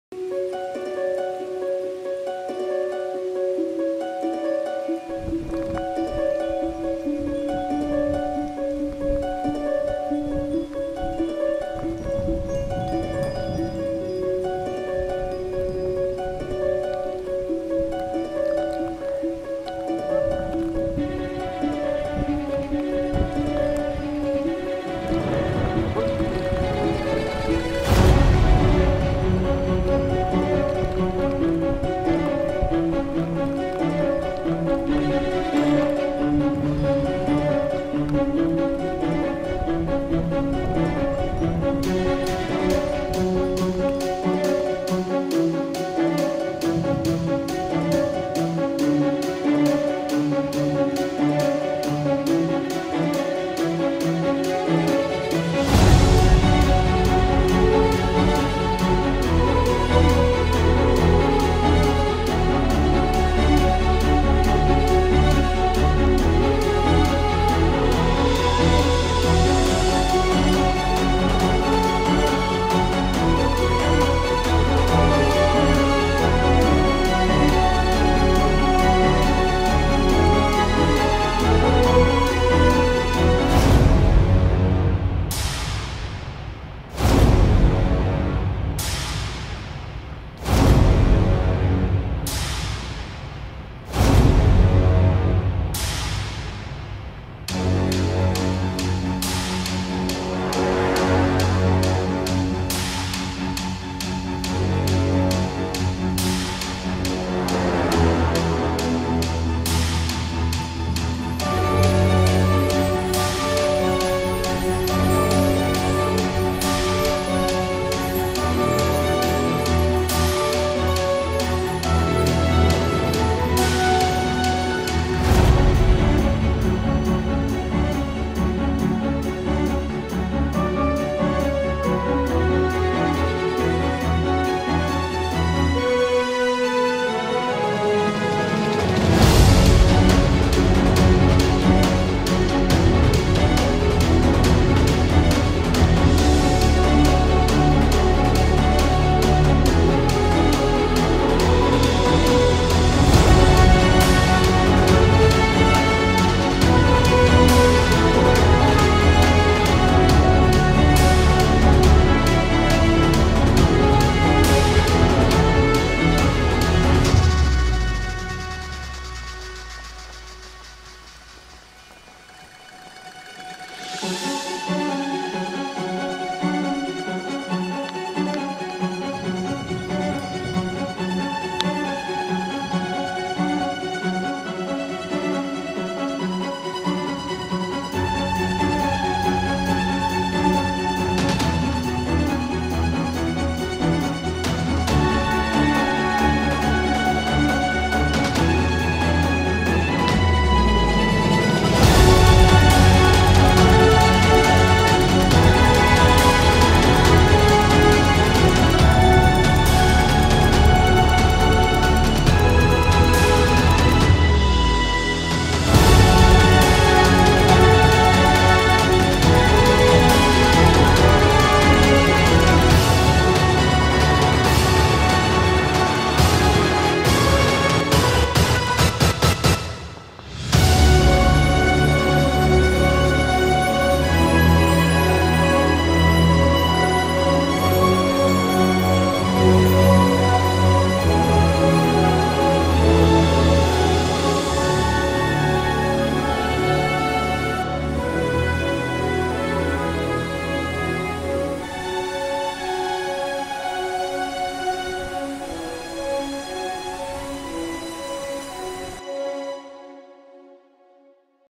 Nevada a Ontinyent. 20 de gener de 2020
Xicotet recull d'imatges de la nevada que va provocar la borrasca Glòria la matinada del 20 de gener de 2020. Com a curiositat, als primers 30 segons es veuen dos llamps que van caure mentre nevada, però pràcticament no es sent el tro, perquè la neu amortigua el so.